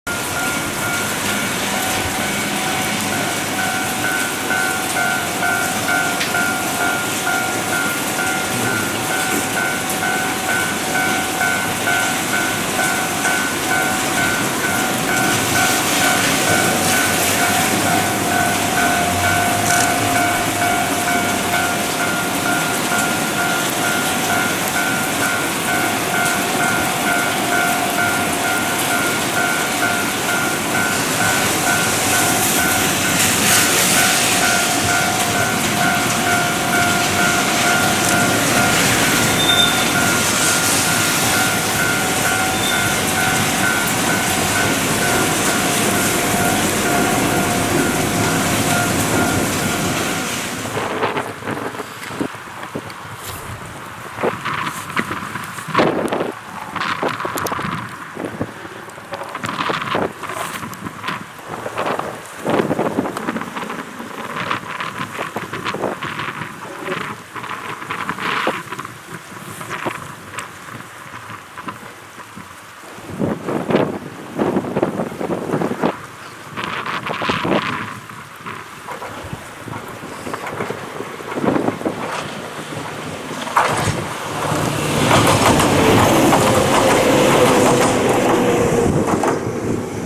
近江鉄道日記 806Ｆ市辺＆103Ｆ蛇溝 （2024.3/26） 巛巛
＊806F：『わたSHIGA』ラッピング（国スポ・障スポ宣伝編成）
＊103F：『彦根』『近江八幡』PR-HM，スノープラウ付，側広告２種